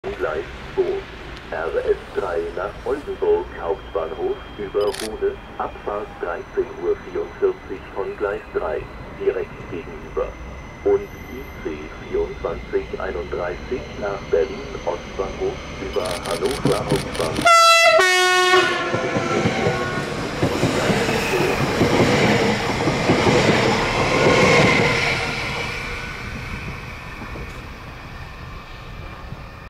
Nordwestbahn Flirt 3428 012 vertrekt in station Delmenhorst + tyfoon (4K)
Nordwestbahn Flirt 3428 012 vertrekt in station Delmenhorst als RS 30 naar Bremen Hbf.

Zaterdag 17 december 2022